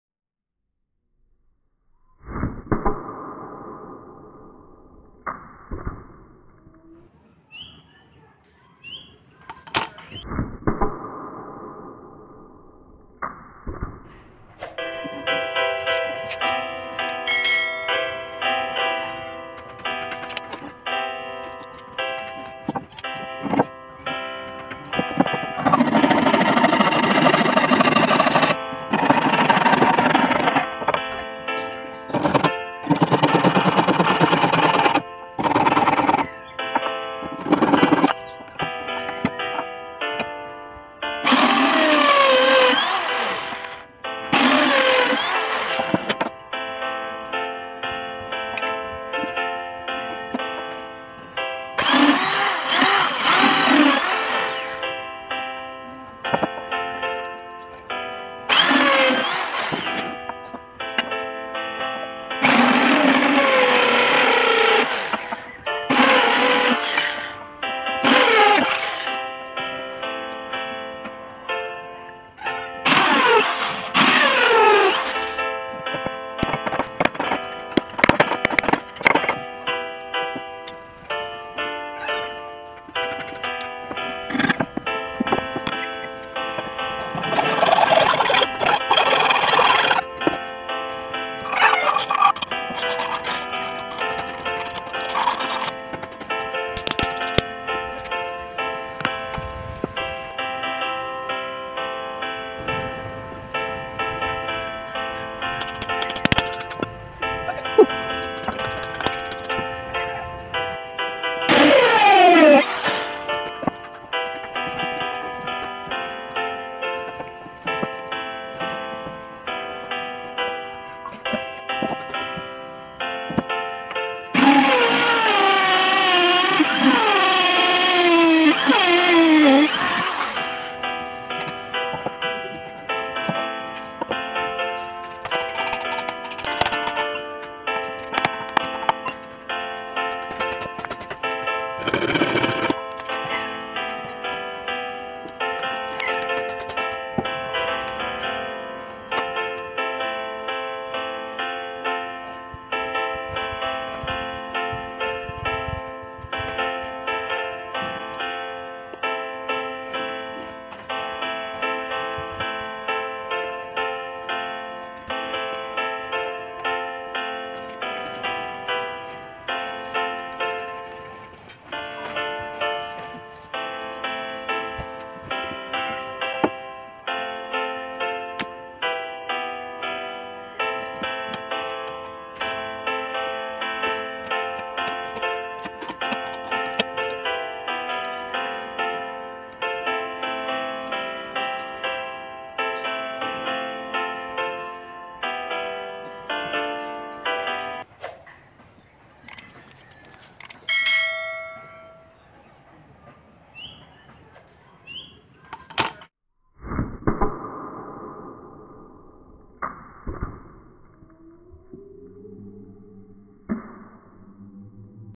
Powerful rubber band sling shot gun || how to make rubber band gun
Welcome everyone to this new video, here you can see how to make powerful sling shot from rubber band, by using wood and some iron metals, see the full video without skipping to know the process of making,

Requirements are, rubber band, wood, animal skin, iron nails and rod of 3mm, hacksaw, hammer, sanding paper, drilling machine, plier, chisel, handsaw, marker pen, measuring tape, screw, etc.